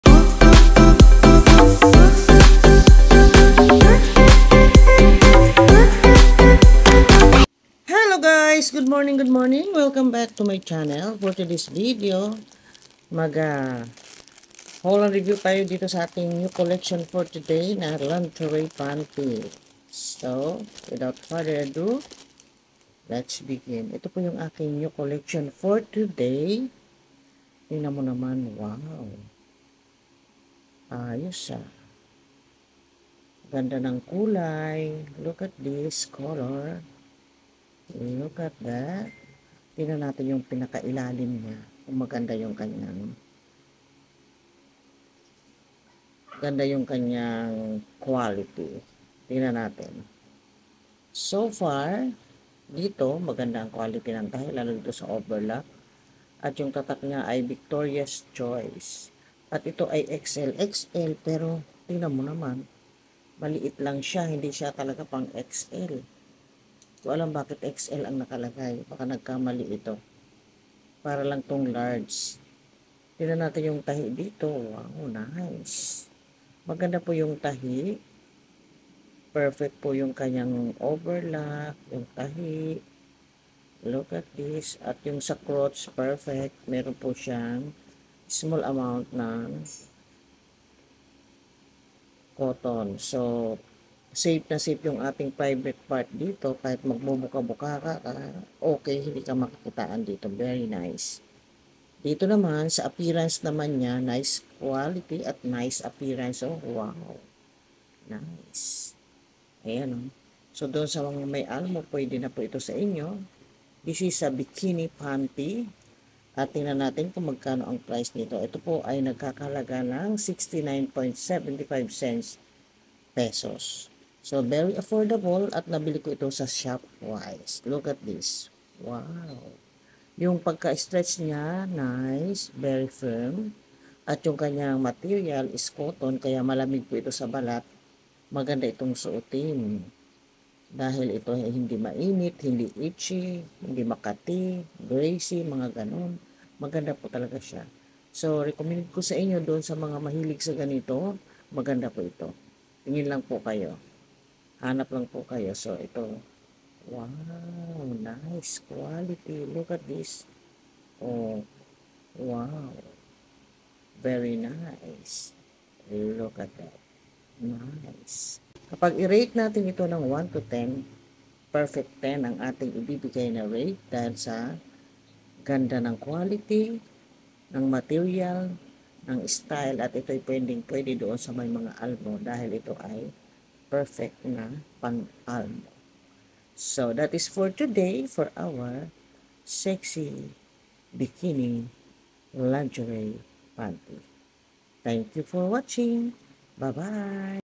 Sexy Bikini Lingerie Elegant Panty from Victoria's Choice Haul and Review #115
This video is not a paid sponsor, this is a haul and Review about the products style, quality, material and very affordable lingerie .